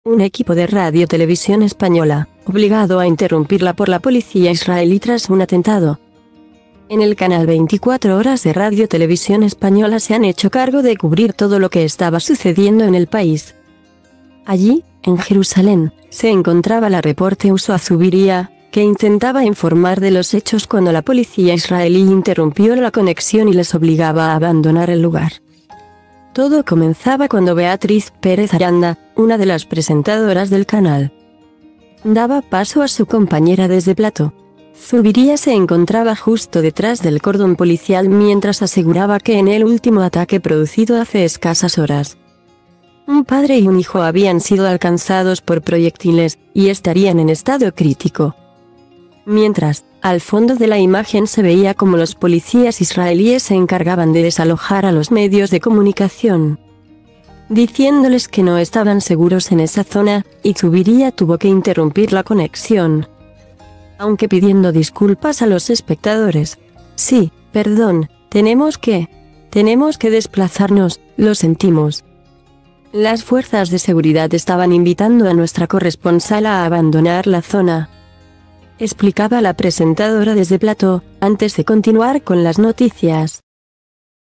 Un equipo de RTVE, obligado a interrumpir la por la Policía israelí tras un atentado
en el Canal 24h de RTVE se han hecho cargo de cubrir todo lo que estaba sucediendo en el país.Allí, en Jerusalén, se encontraba la reporte Usoa Zubiría, que intentaba informar de los hechos cuando la Policía israelí interrumpió la conexión y les obligaba a abandonar el lugar. Todo comenzaba cuando Beatriz Pérez Aranda, una de las presentadoras del canal,